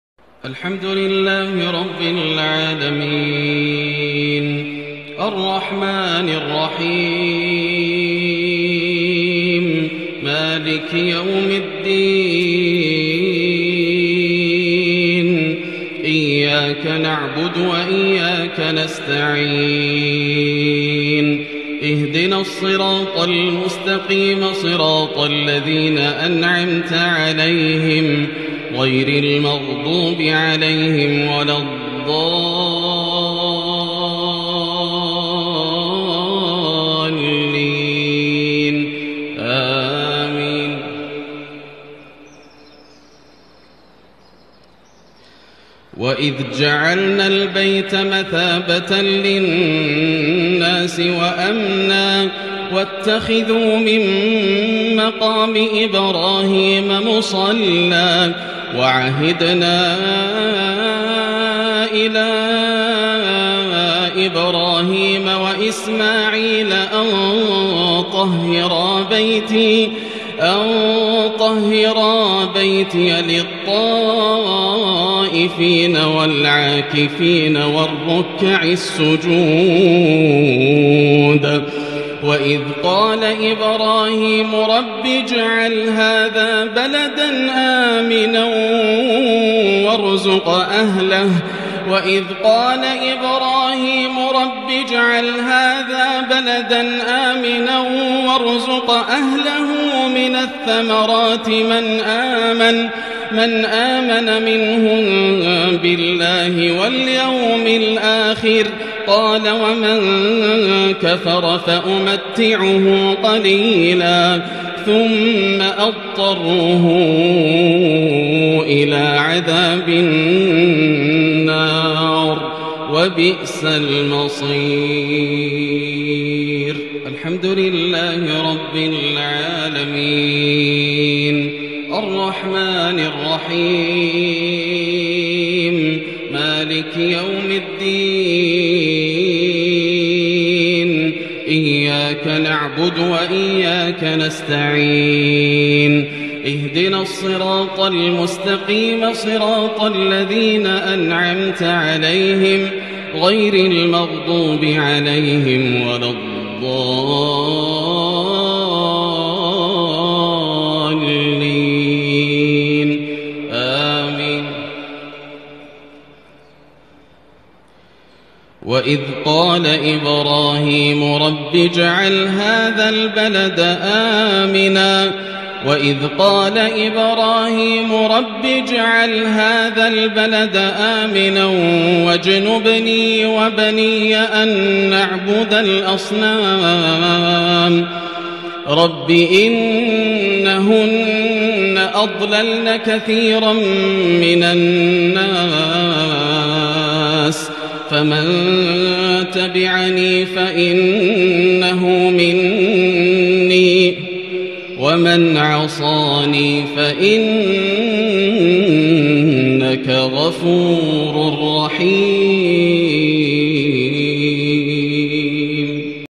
بركة دعوة إبراهيم عليه للبلد الحرام حيث جعله الله مكانا آمنا للناس وتفضل على أهله بأنواع الأرزاق
#﴿وإذ قال إبراهيم رب اجعل هذا بلدا آمنا وارزق أَهلَهُ مِنَ الثَّمَراتِ مَن آمن منهم بالله و اليوم الآخر#تلاوة فضيلة الشيخ د.ياسر_الدوسري من صلاة العشاء 15 محرم 1442هـ#beautiful